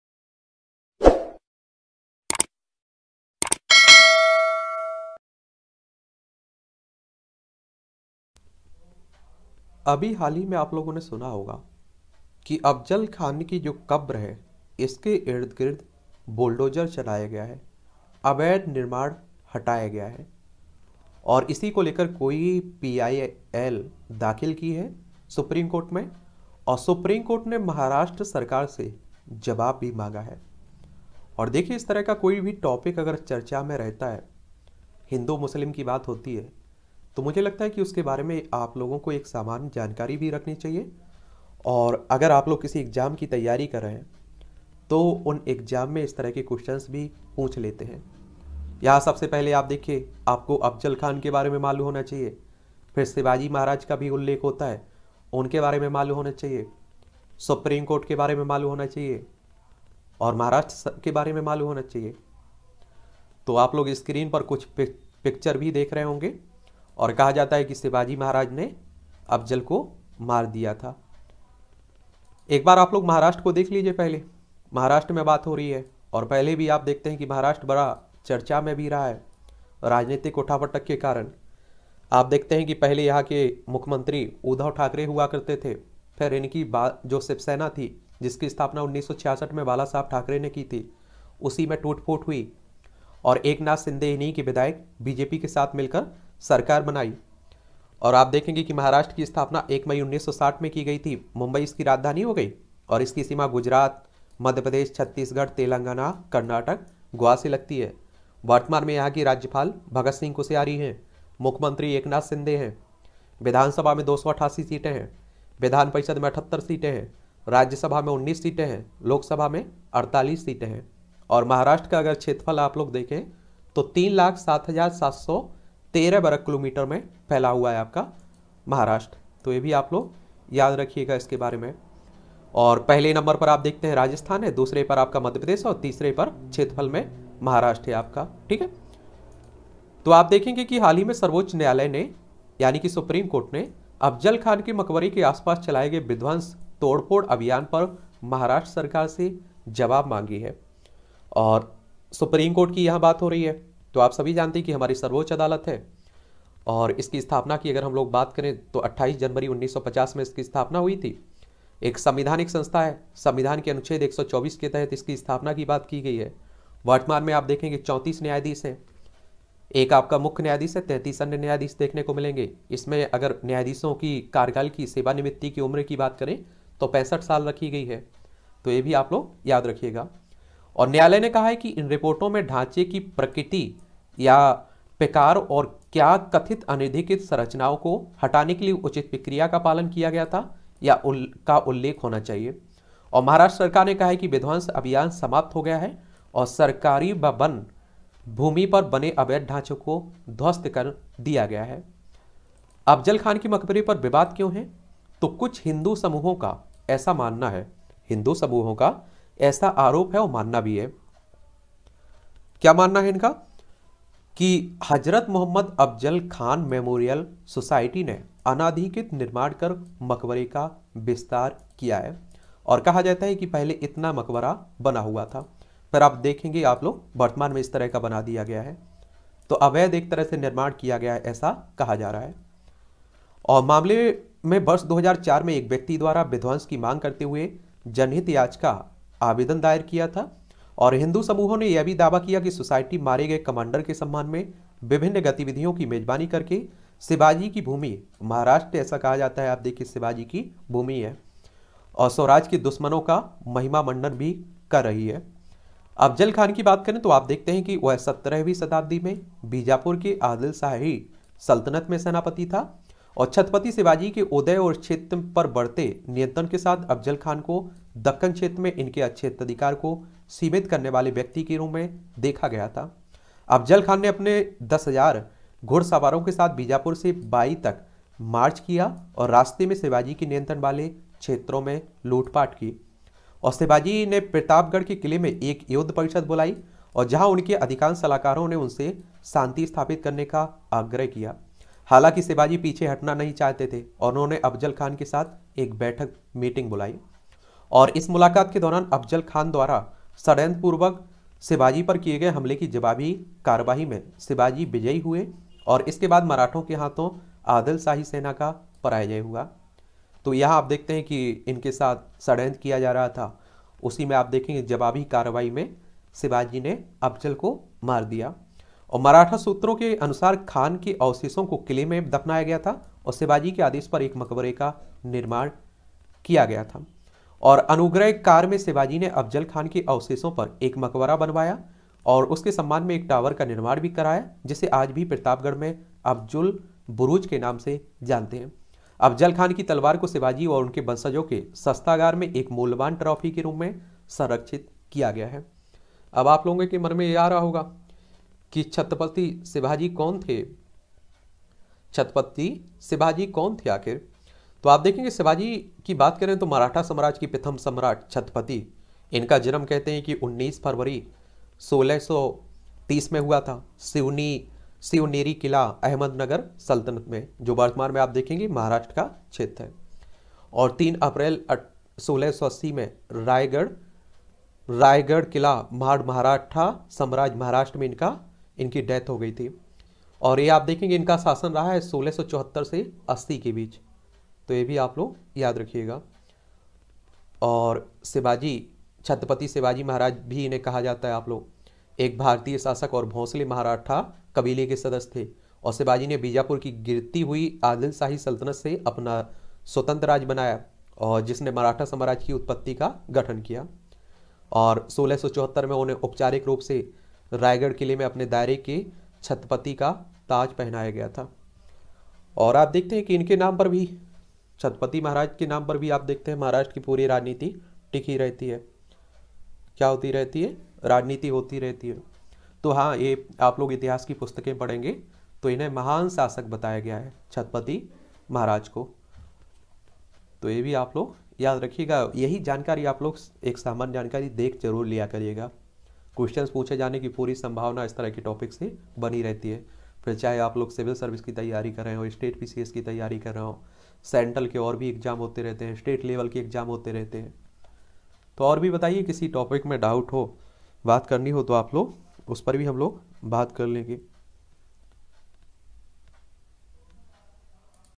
अफजल खान || Afzal Khan || अफज़ल खान का मकबरा || Afzal Khan's Mausoleum
हाल ही में सर्वोच्च न्यायालय (SC) ने अफज़ल खान के मकबरे के आसपास चलाए गए विध्वंस/तोड़फोड़ अभियान पर महाराष्ट्र सरकार से रिपोर्ट मांगी है।